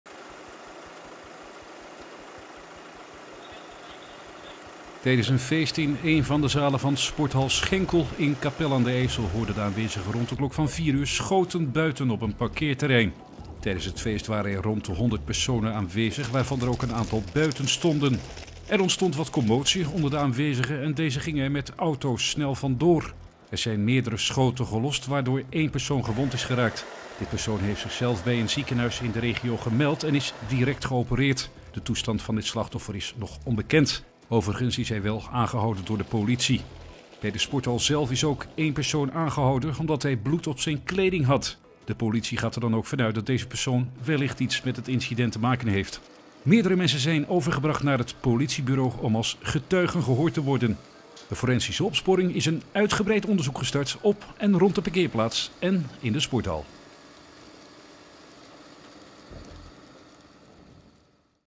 Schietpartij bij sporthal Schenkel
Voice-over: Ed Koorengevel - Camera: Video Duivestein

CAPELLE A/D IJSSEL - Tijdens een feest in één van de zalen van sporthal Schenkel in Capelle aan den IJssel hoorde de aanwezigen rond de klok van vier uur schoten buiten op de parkeerplaats. Op het feest waren rond de honderd personen aanwezig, waarvan een aantal buiten stonden. Er ontstond wat commotie onder de aanwezigen en deze gingen er met hun auto's snel van door.

Er zijn meerdere schoten gelost, waardoor één persoon gewond is geraakt. Deze persoon heeft zichzelf bij een ziekenhuis in de regio gemeld en is direct geopereerd. De toestand van dit slachtoffer is verder onbekend. Hij is wel aangehouden door de politie.

Bij de sporthal zelf is ook één persoon aangehouden, omdat hij bloed op zijn kleding had. De politie gaat ervan uit dat deze persoon wellicht iets met het incident te maken heeft.

Meerdere mensen zijn overgebracht naar het bureau om als getuige gehoord te worden.

De forensische opsporing is een uitgebreid onderzoek gestart op en rond de parkeerplaats en in de sporthal.